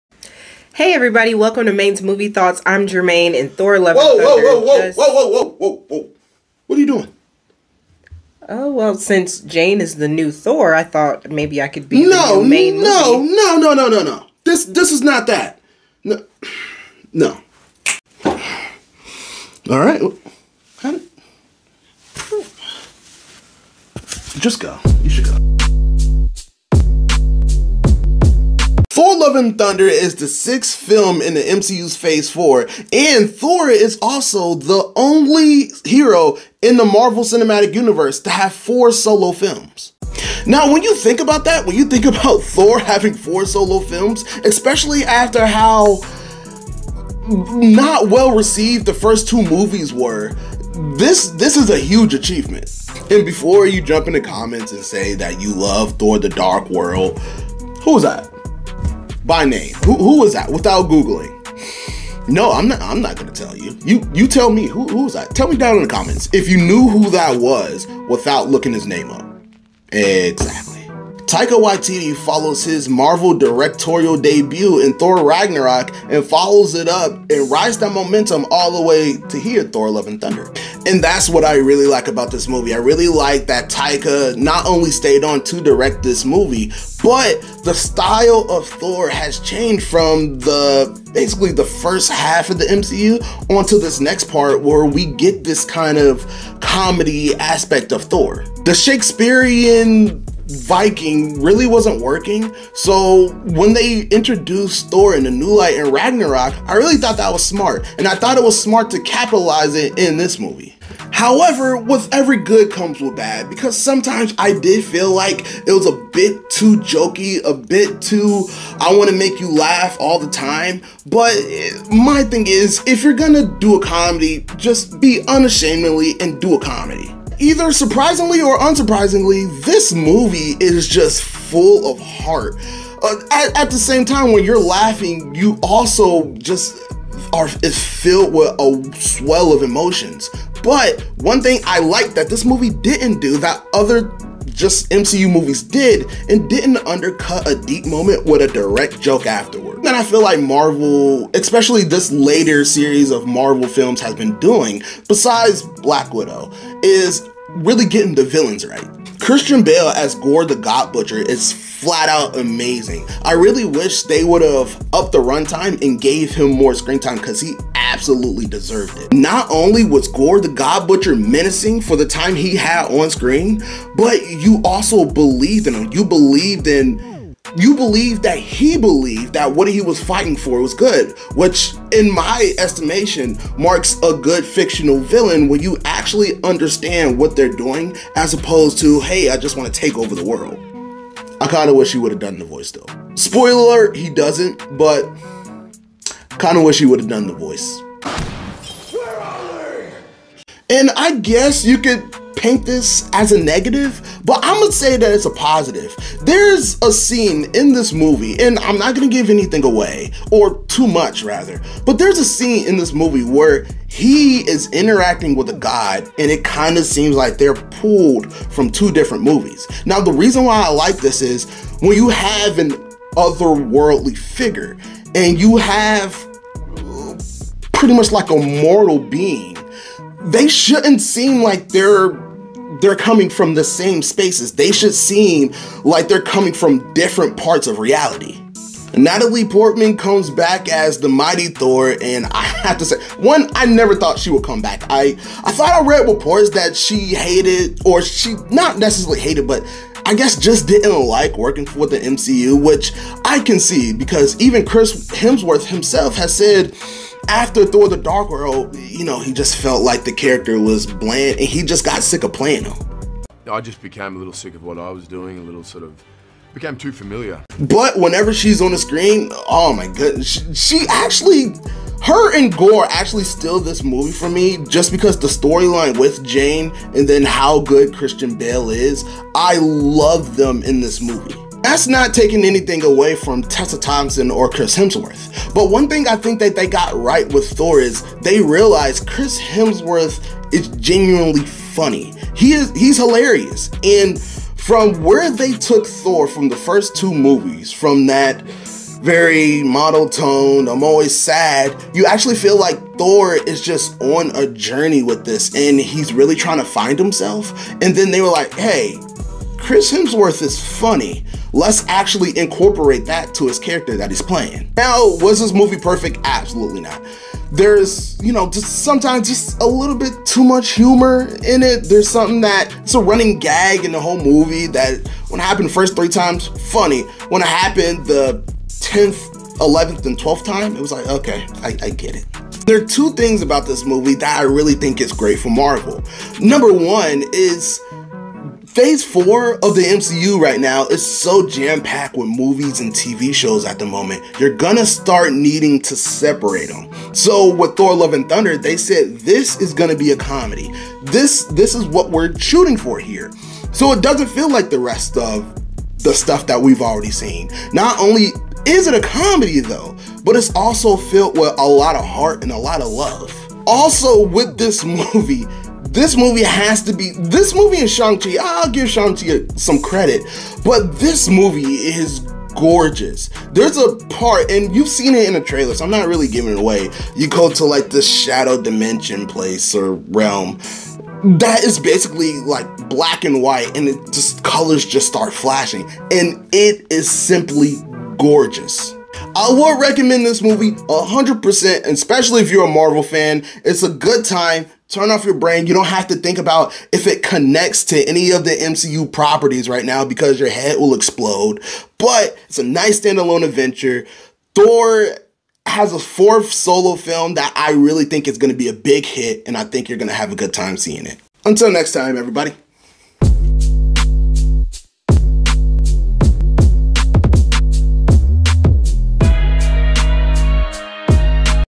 Why Thor: Love and Thunder is great! - Movie Review
Sometimes when you say you can turn your brain off and have a good time, many people see that as a bad thing. But with everyone trying to connect the dots with Marvel's Phase 4 sometime turning your brain off and not thinking ahead is a good thing.